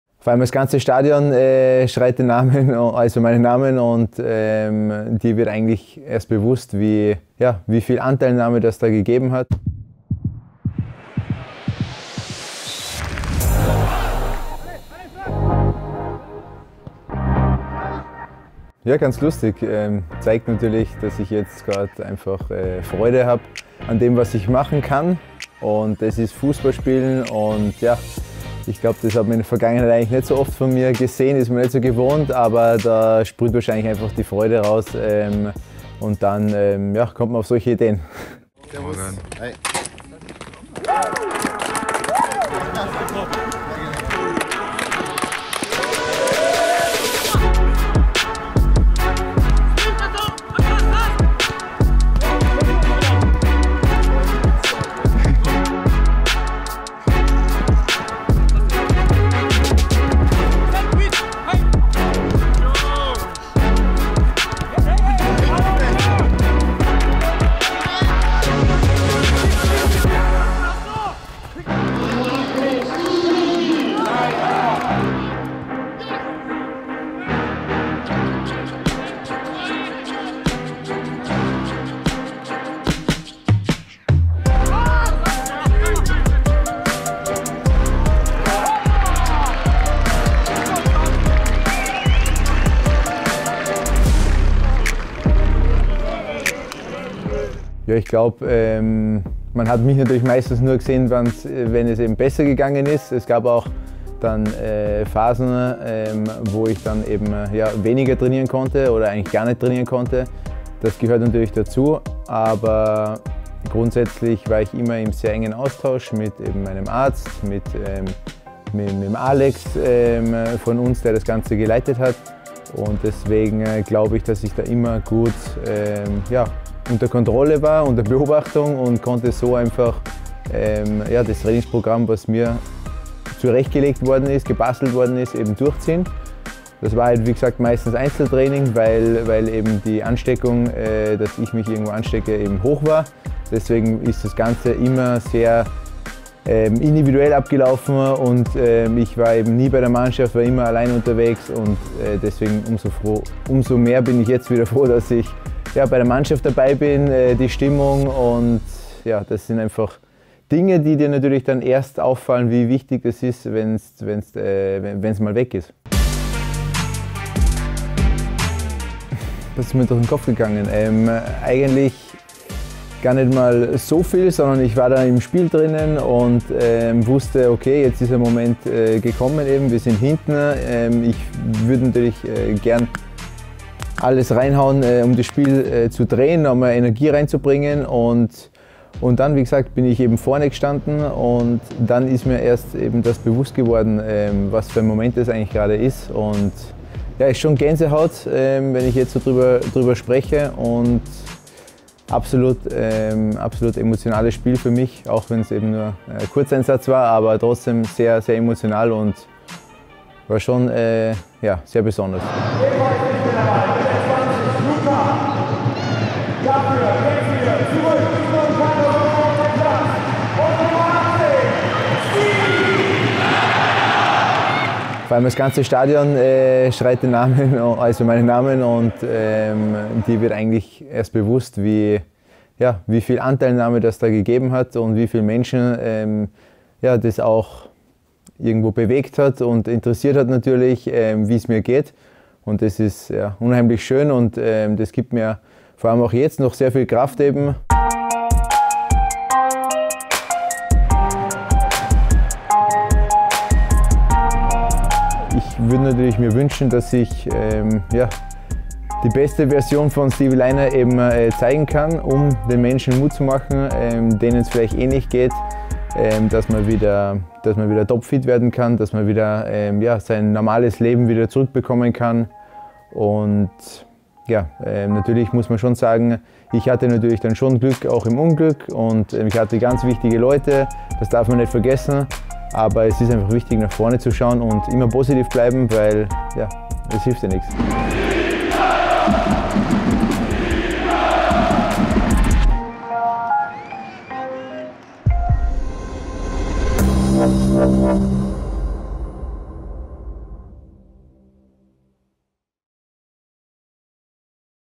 Welcome back, Stevie! 💚 | FohlenInsight
Es ist die 71. Minute im Spiel gegen den FC Augsburg als der BORUSSIA-PARK sich erhebt und es besonders laut wird! Der Grund: Stevie Lainer steht an der Seitenlinie bereit, um nach seiner überstandener Krebserkrankung eingewechselt zu werden. Wir haben mit ihm über diesen besonderen Moment gesprochen!

Schön, dass du wieder da bist, Stevie!

🐎🎥 Du willst noch mehr exklusiven Inhalt rund um die Fohlen? Dann schau mal bei tv.borussia.de vorbei!